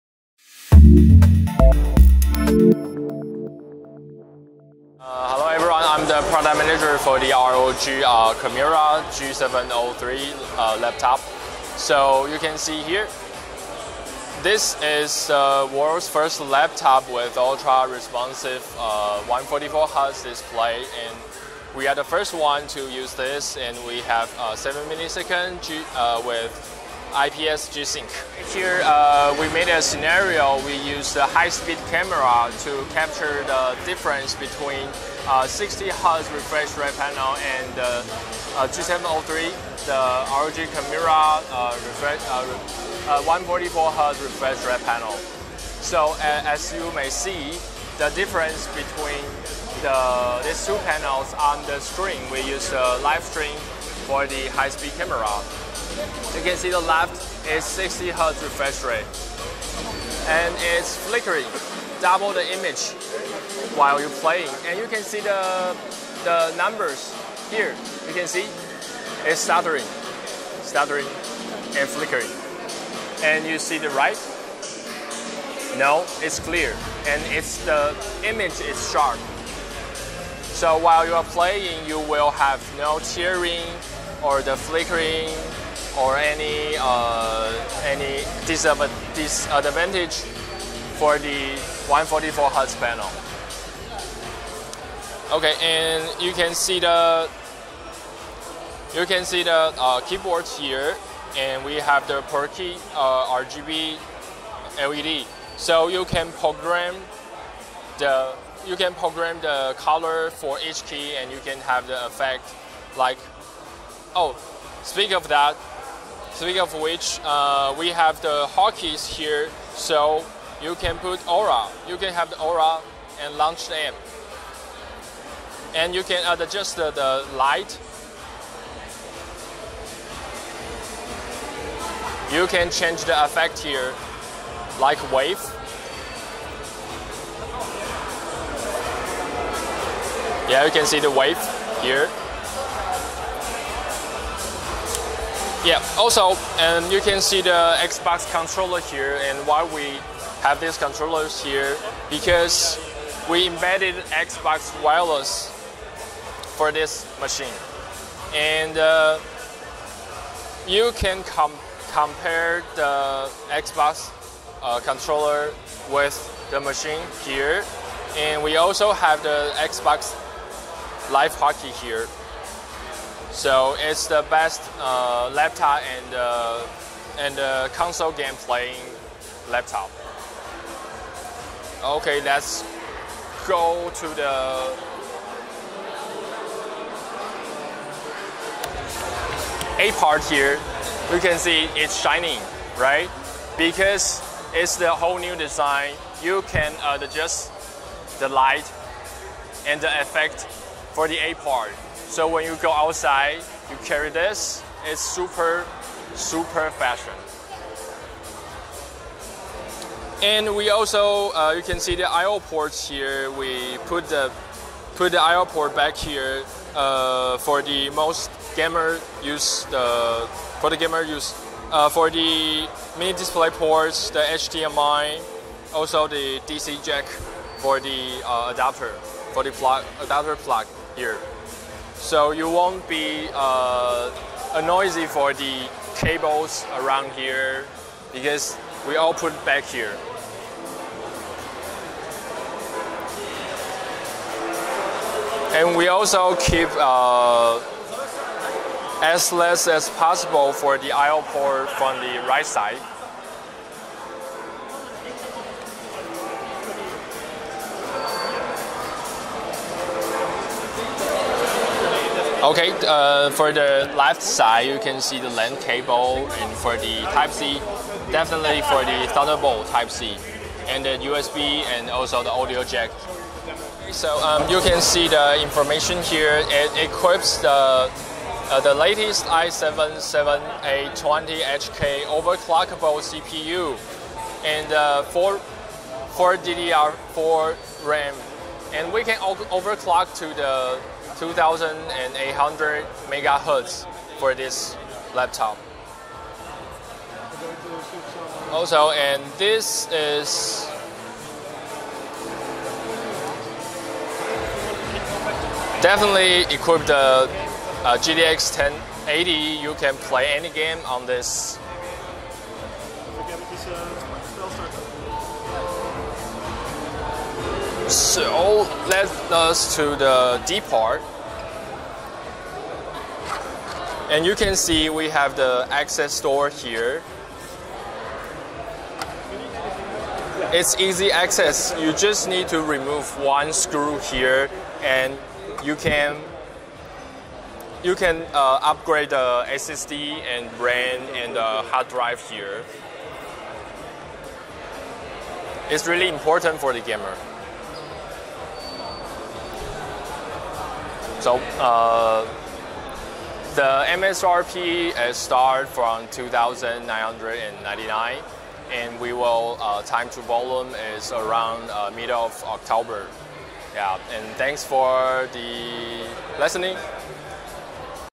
ASUS ROG Chimera G703 gaming notebook
THIS GAMING PC COMES WITH INTEL CORE KABYLAKE CPU,GEFORCE GTX 1080,SSD PERFORMANCE AND 17.3" FULL HD IPS DISPLAY.